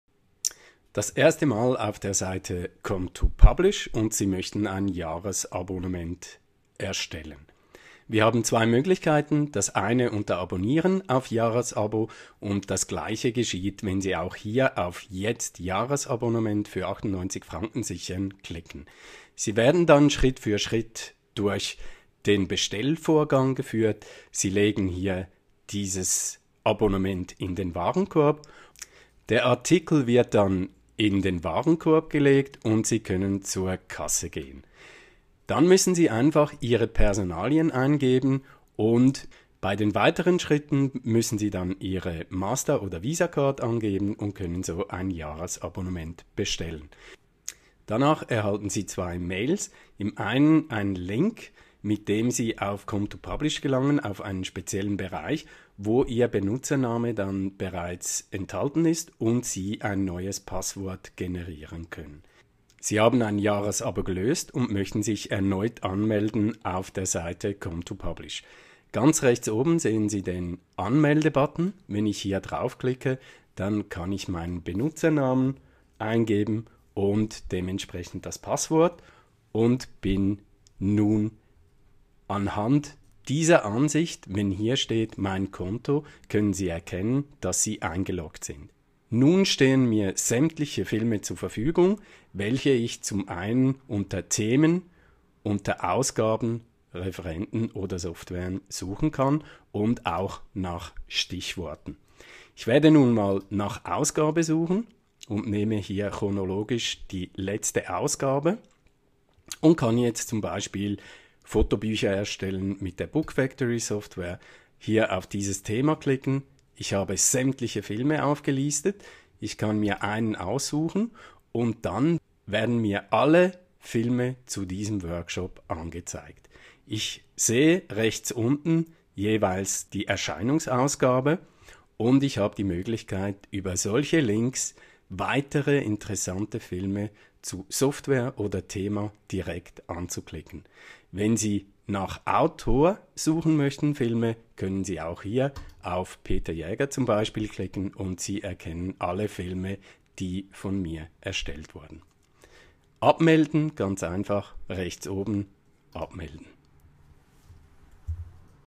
So funktioniert com2publish, die geniale Lernplattform für Medienproduzenten und Corporate Publisher
Aus der Praxis und für Praktiker. Lerninhalte und Experten-Netzwerk zentral an einem Ort. Jahresabonnement nur 98 Franken.